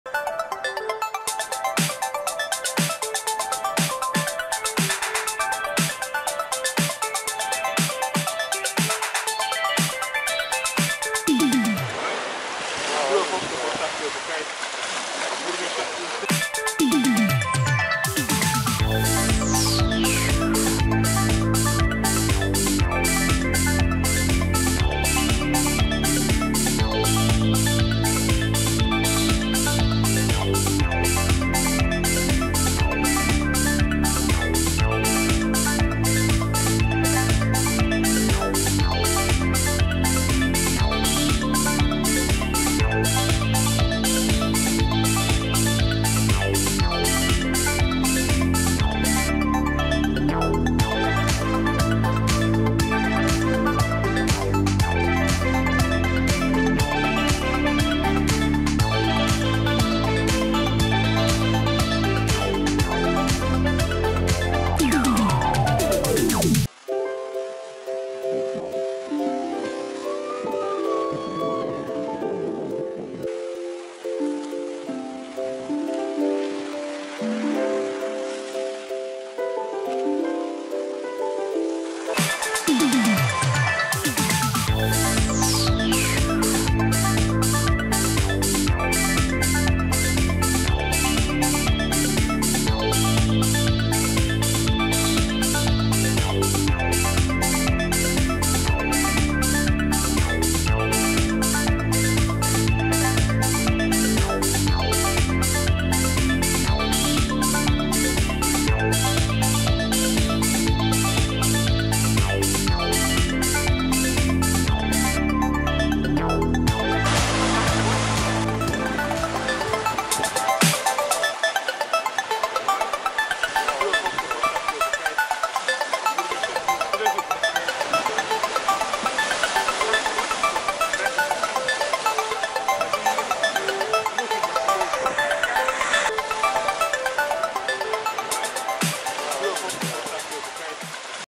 Beach Mogren New (пляж Могрен новое)
Пляж Могрен самый красивый и интересный пляж в Черногории. Новое видео с пляжа Могрен в Будве! Чистое Адриатическое море. Красивые скалы Черногории. Всё это пляж Могрен в Будве.
Mogren Beach is the most beautiful and interesting beach in Montenegro. New video from Mogren beach in Budva! Pure Adriatic Sea. Beautiful cliffs of Montenegro. All this is Mogren Beach in Budva.